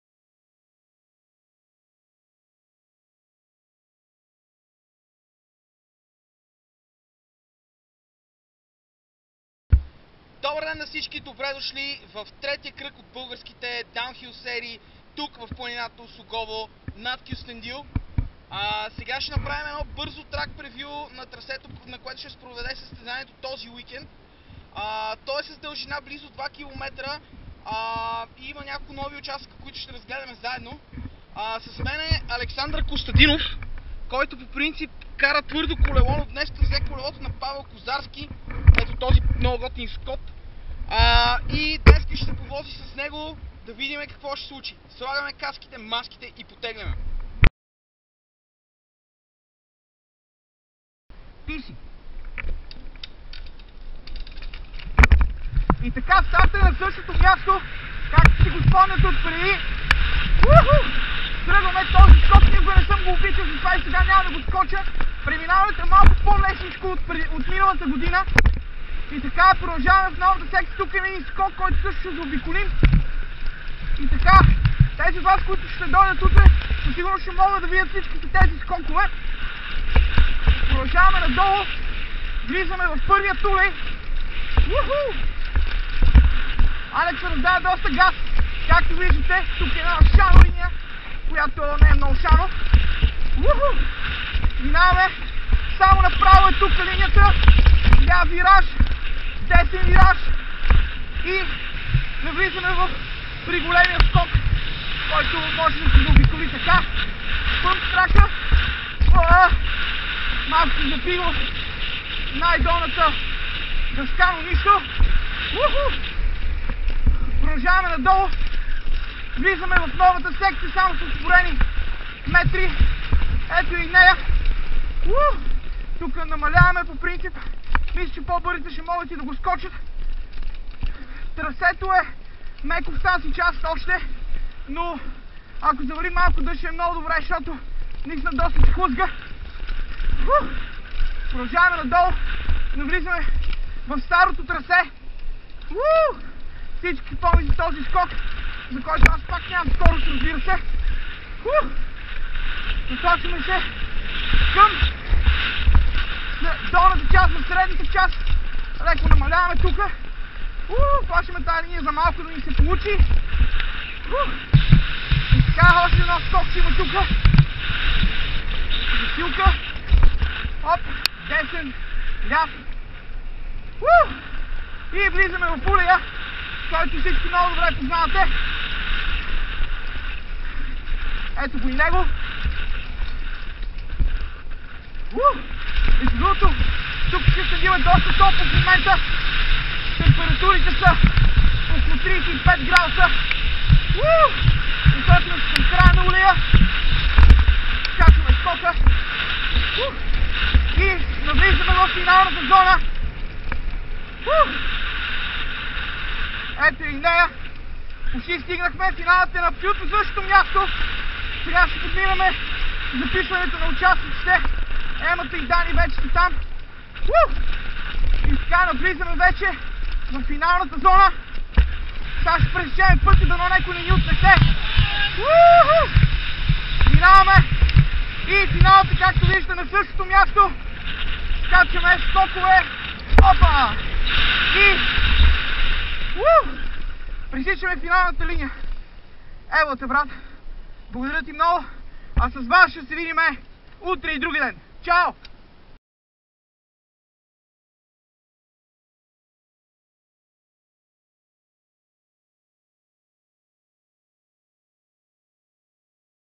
BG Downhill Series 2017 - Round 3 - Osogovo DH Cup
Оглед на трасето за третия кръг от Българските Серии по Спускане в Осогово. По трасето сме с Александър Костадинов, който кара велосипеда на Павел Козарски.